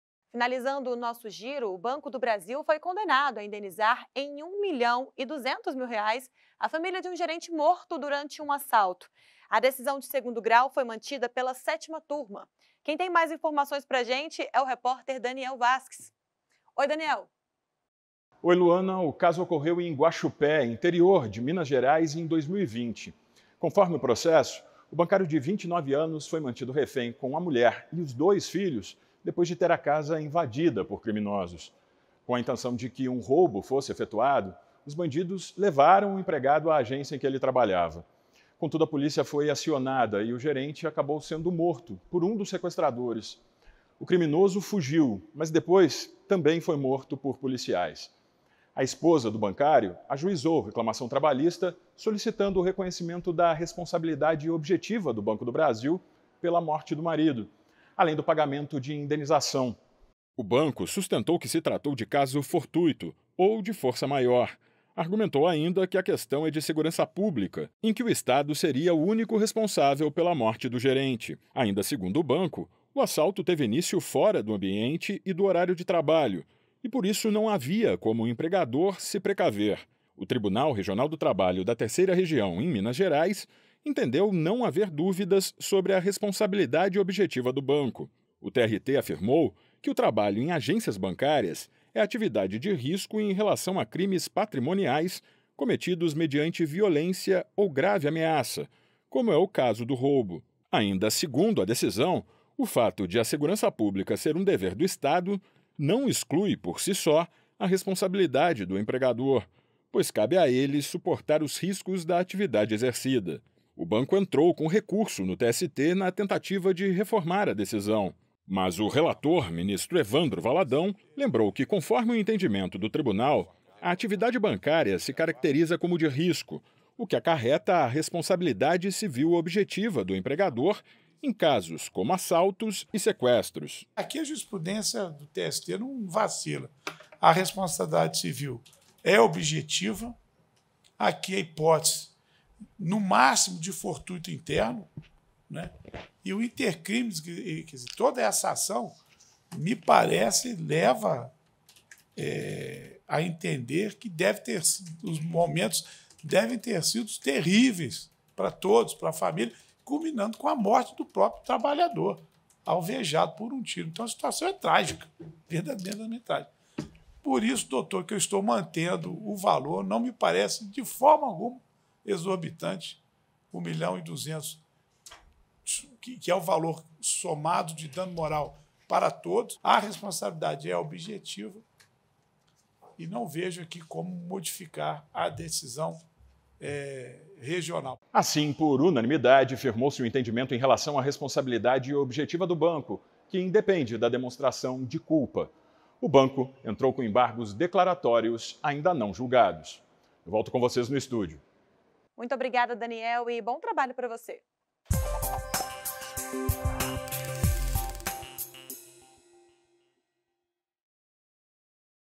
Banco terá de indenizar família de gerente executado durante assalto
A Sétima Turma do Tribunal Superior do Trabalho manteve decisão que condenou o Banco do Brasil S.A. a pagar R$1,2 milhão de indenização à família de um gerente morto em assalto com um tiro na cabeça na porta da agência do banco em Guaxupé (MG).

Por unanimidade, firmou-se o entendimento em relação à responsabilidade objetiva do banco, que independe da demonstração de culpa.